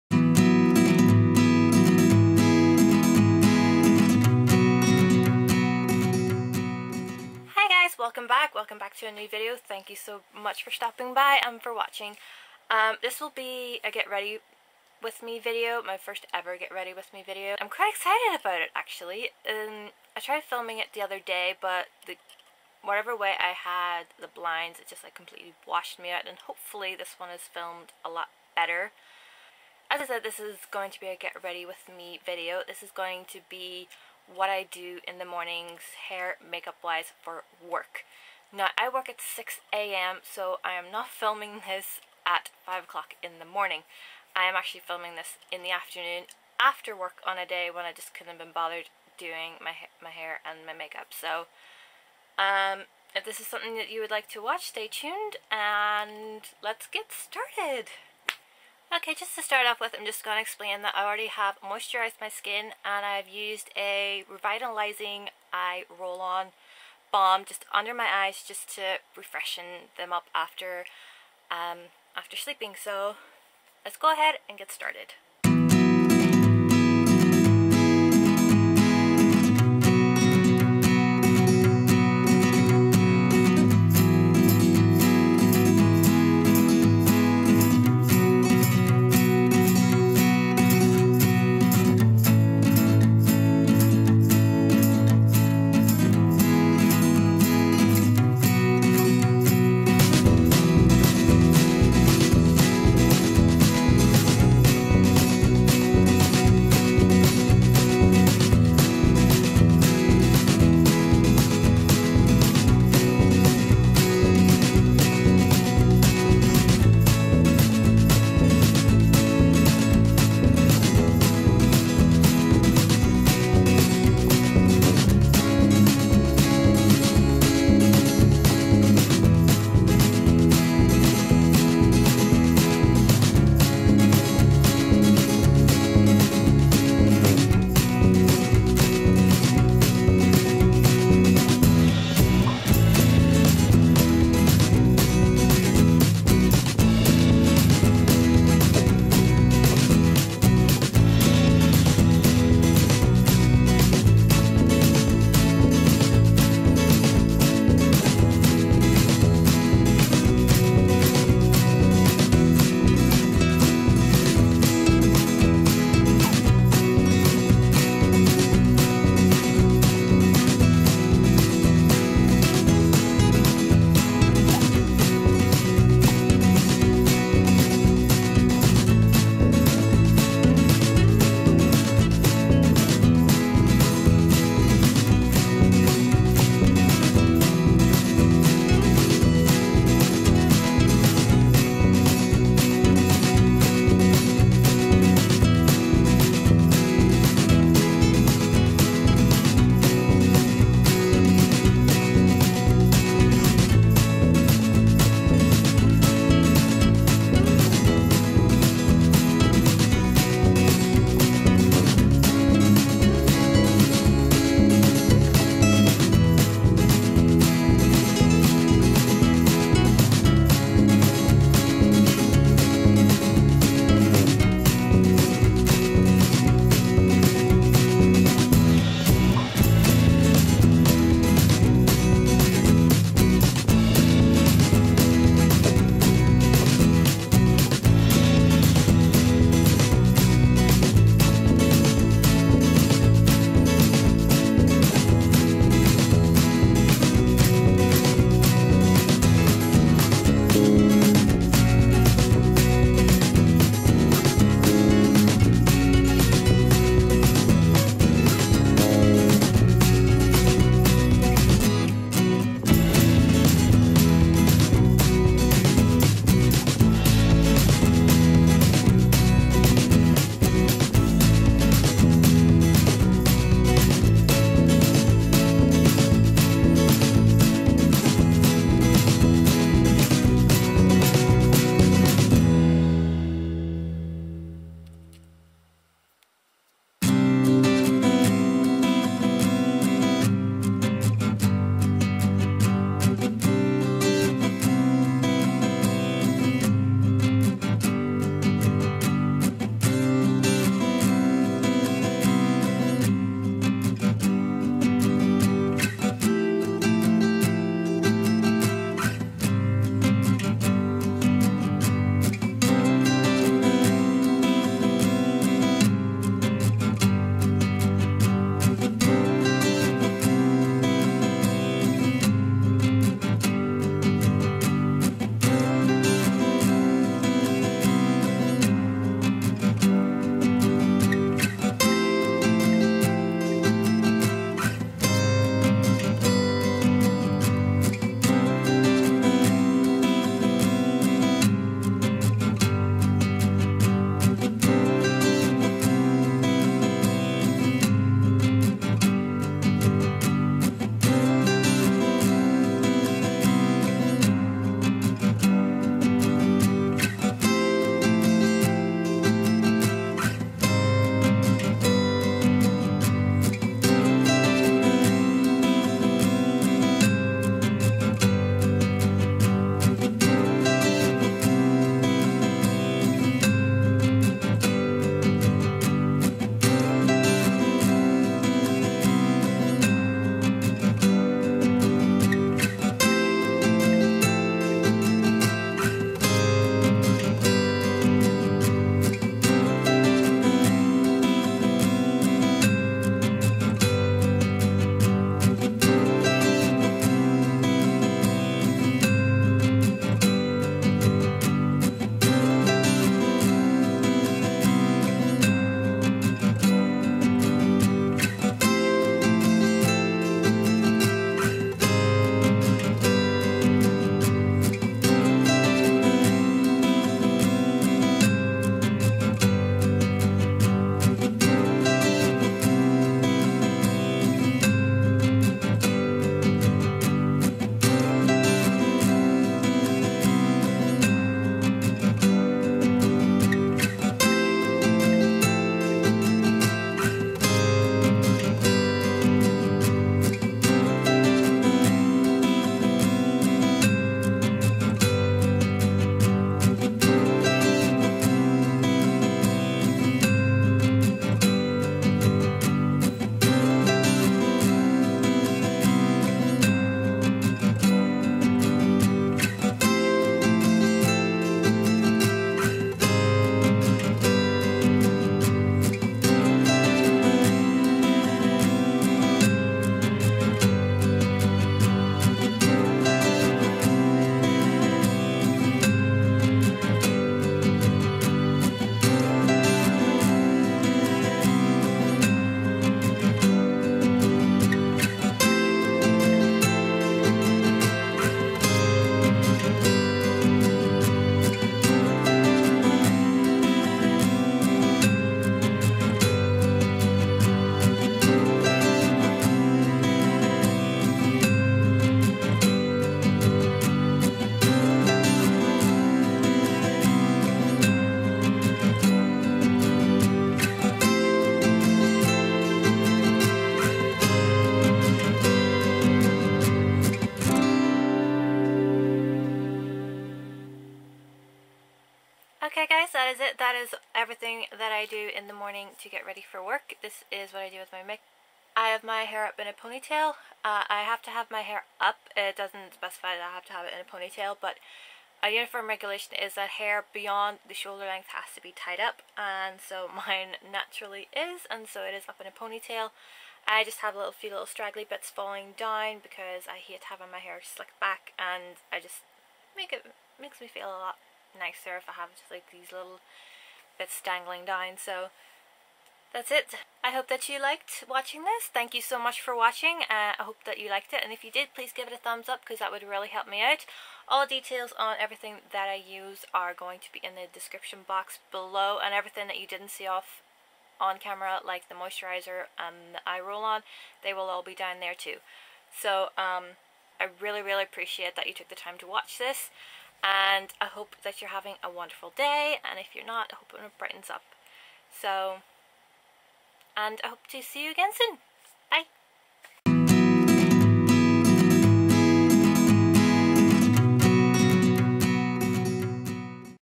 Get Ready With Me - Work Edition | kazzified29
Welcome everyone to my morning make-up and hair routine for work.

- - - - LOOK BELOW FOR MORE INFORMATION - - - -

Where I work has a uniform regulation for hair and so my hair is up in a pony tail 90% of the time for simplicity and ease. From time to time, I may decide to style it in a different way. When filimg, I was being lazy. =)

MAKE-UP USED:
Simple Kind to Skin Replenishing Rich Moisturiser
Simple Kind to Eyes Revitalising Eye Roll-On
Rimmel Fix & Perfect Pro Primer - 002
L'Oreal Lumi Magique Highlighting Pen - 1 Light
Rimmel Hide the Blemish Concealer - 001 Ivory
Rimmel Match Perfection Foundation - Light Porcelain
Too Faced Shadow Insurance Eye Shadow Primer - Candlelight
Maybelline 24hr Color Tattoo - 65 Pink Gold
Maybelline The Rocket Mascara
Maybelline Expression Kajal eyeliner - White
Rimmel Stay Matte  Pressed Powder - 001 Transparent
Urban Decay Naked Flushed palette - Streak
The Body Shop Honey Bronzer- 02 Fair matte
NYX Butter Gloss - BLG05 Creme Brulee

TOOLS USED;
Real Techniques make up brushes
- Buffing Brush
- Detailer Brush
- Powder Brush
- Retractable Kabuki Brush
- Blush Brush

Forever 21 Shadow Brush

Ted Baker eyelash curler